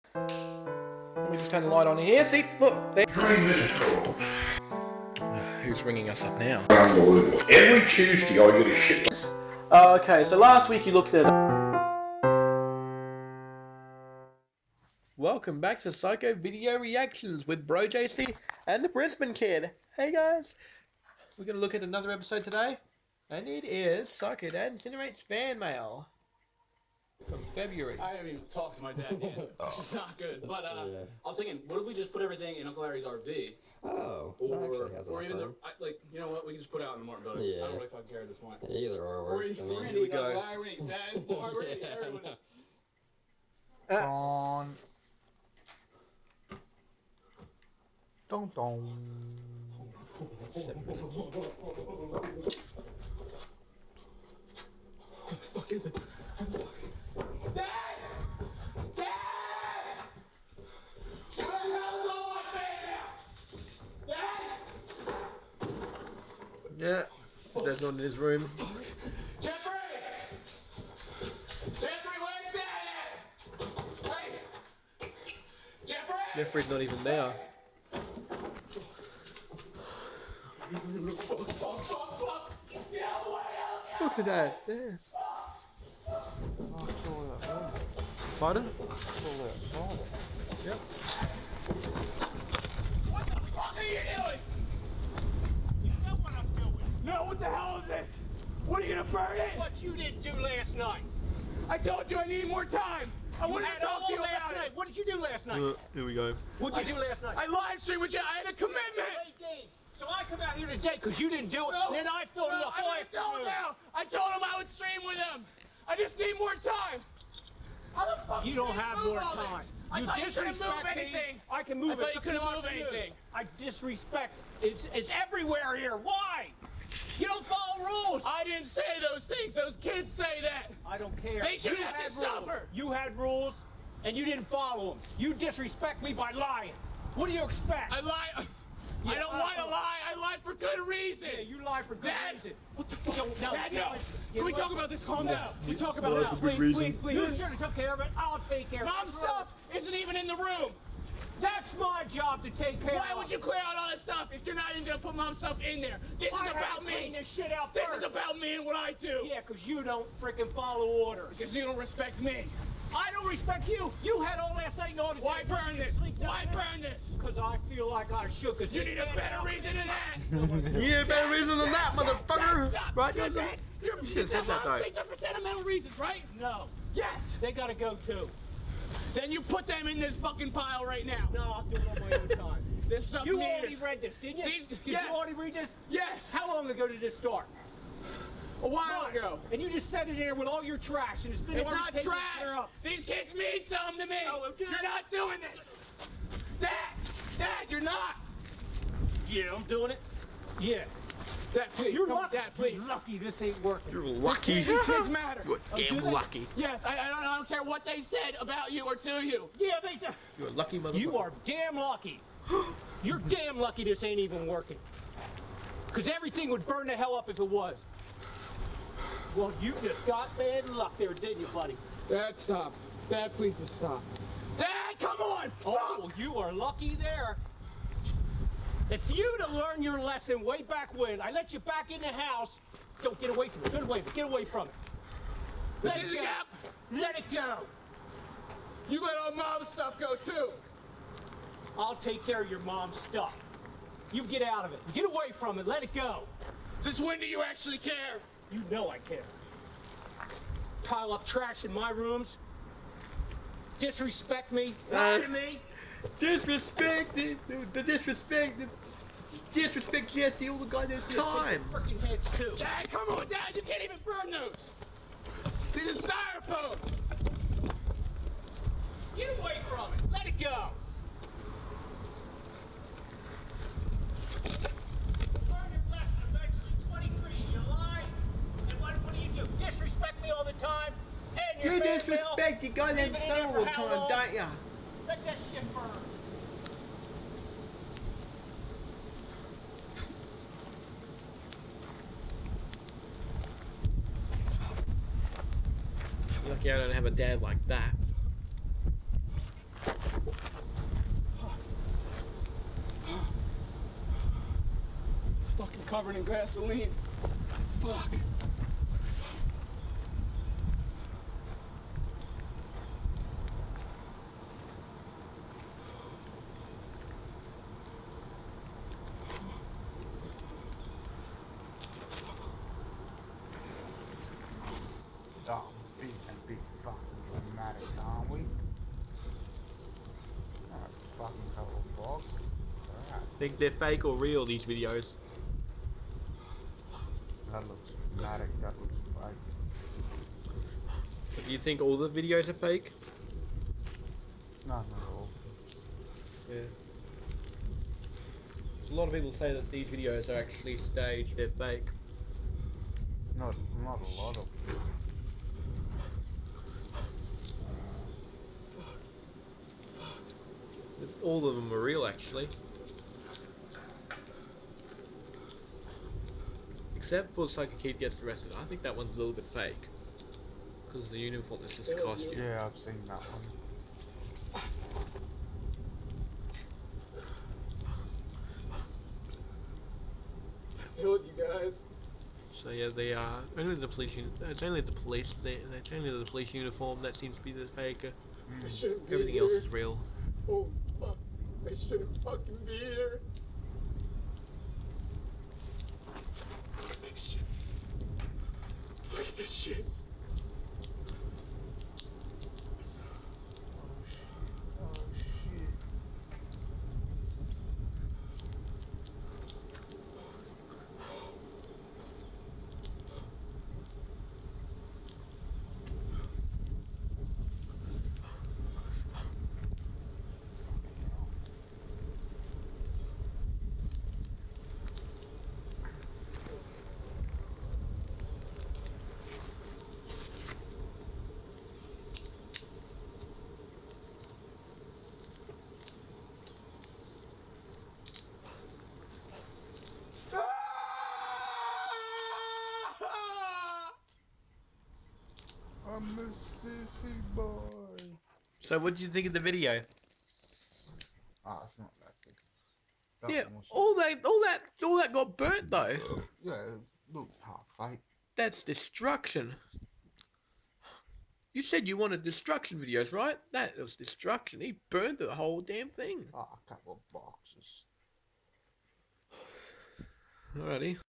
Psycho Brother Reactions Video - Season 1, Episode 26 (HD)
Finally! Episode 26 is here. Welcome to the 26th episode of Psycho Brother Reactions Video. In this episode, we look at: Psycho Dad Incinerates Fan Mail. This was taken in May 2016 and Jason assumes in this video that this psycho video is "dramatised" (fake). Jason is seeing this video as the August video (the first ever recorded reaction video from him) gave me an idea to start this series so I filmed this.  Enjoy the 26th episode. Episode 27 will be out in February. Made with Magix Movie Edit Pro 2014 Premium. It's the only episode I made at my dad's house while I was on holiday there in November.

I will be releasing two episodes of Psycho Brother Reactions every week.